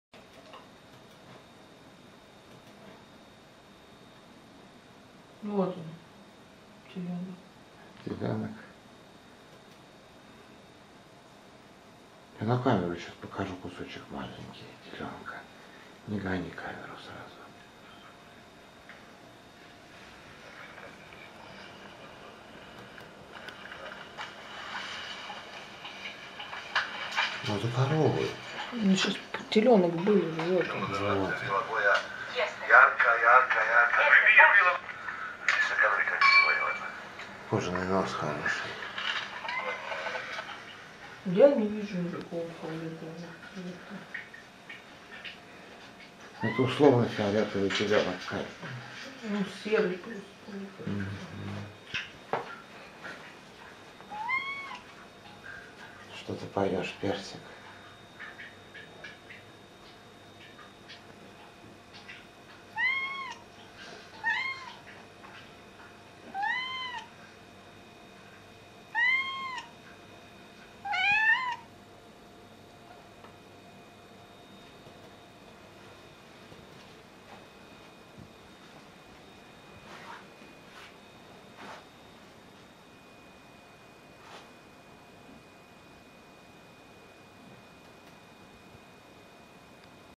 01-23-2012 - утро (фиолетовый телёнок и кот Персик)
Москва, 2012

жена нашла в сетке репортаж о фиолетовом телёнке... и кота потянуло общаться... случайно снялось... кухонька нашей квартирки...

Каля, я люблю тебя...